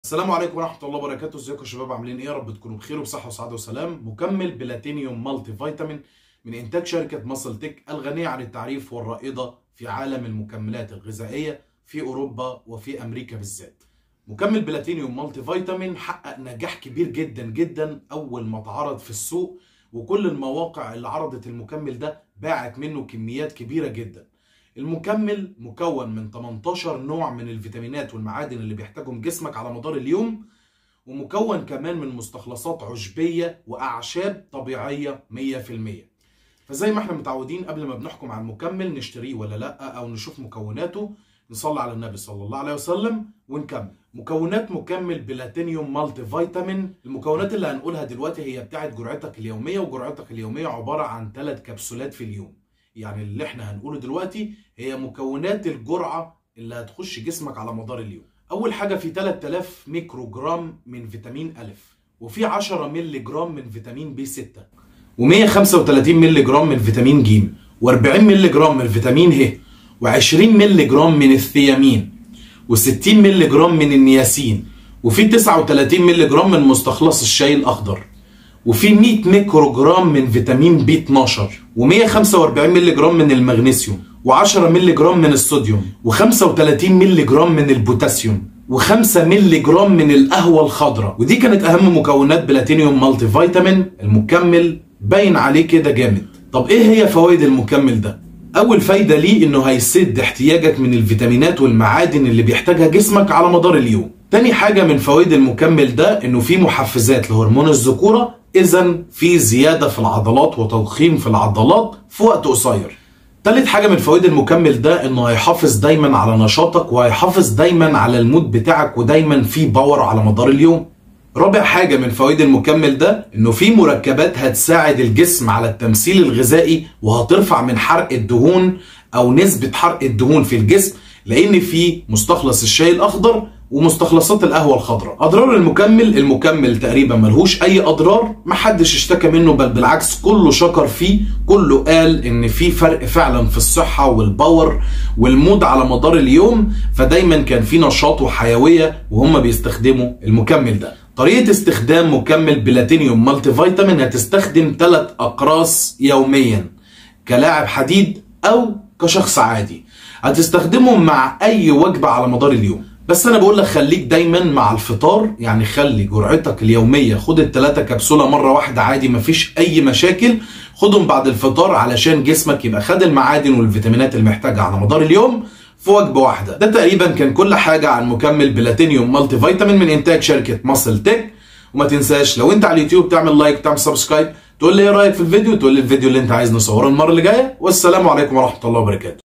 مكمل بلاتنيوم مالتي فيتامين مكونات طريقة استخدام بالتفصيل
مكمل بلاتنيوم مالتي فيتامين انتاج شركة ماصل تك مكمل بلاتنيوم مالتي فيتامين مكون من ١٨ نوع من الفيتامينات والمعادن المهمه جدا للاعب كمال الاجسام و الشخص العادي واي رياضة اخري بلاتنيوم مالتي فيتامين يحتوي علي الشاي الأخضر والقهوه الخضراء مما يحفظ عملية التمثيل الغذائي ورفع معدل حرق الدهون في الجسم اثناء الدايت و التنشيف و انقاص الوزن مكمل قوي جدل لك كلاعب كمال اجسام ومن المكملات الغذائيه المهمه ل تضخيم العضلات بسرعه لانه يرفع من معدل هرمون الذكوره

كل ما يخص لعبة كمال الاجسام تغذيه وتمرين ومكملات ونصائح للمبتدئين